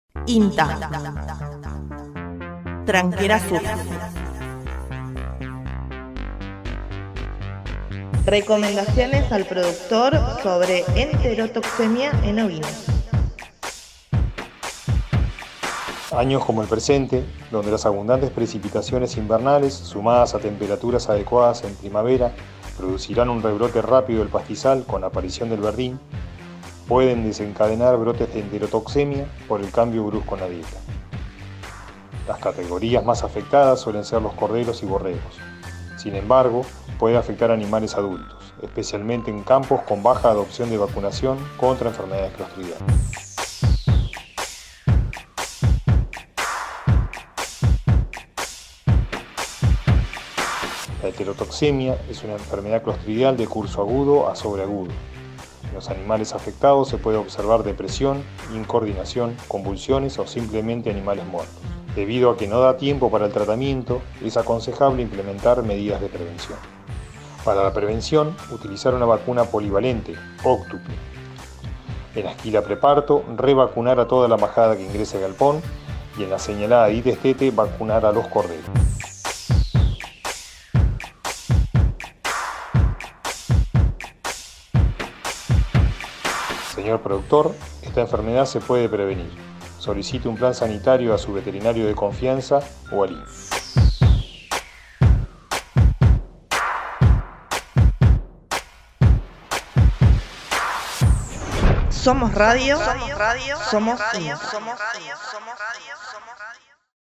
INTA Santa Cruz: Enterotoxemia en ovinos
El Médico Veterinario Marcelo Aguilar, integrante de la Agencia de Extensión Rural Puerto San Julián, nos acerca recomendaciones para el sector productivo sobre la enterotexemia en ovinos.